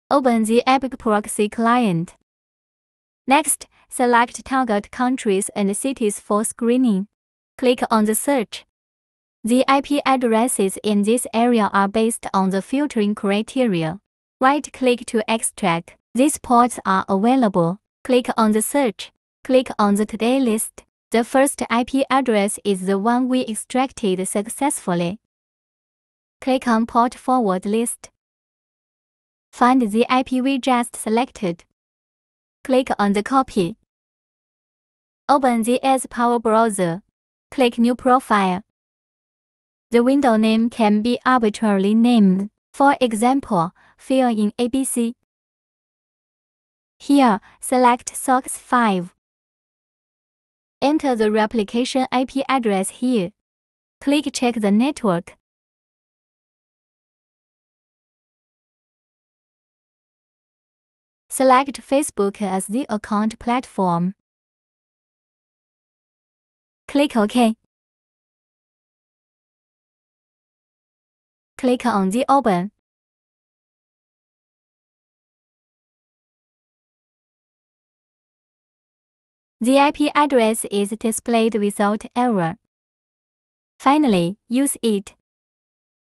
Tutorial of ABC S5 Proxy at AdsPower Browser
ABCproxy，the cheapest Socks5 proxy.
100% rael residential IP.
Time-limited discount bring you the lowest price for the whole network.
Check this video to figure out how to use ABCproxy at AdsPower Browser.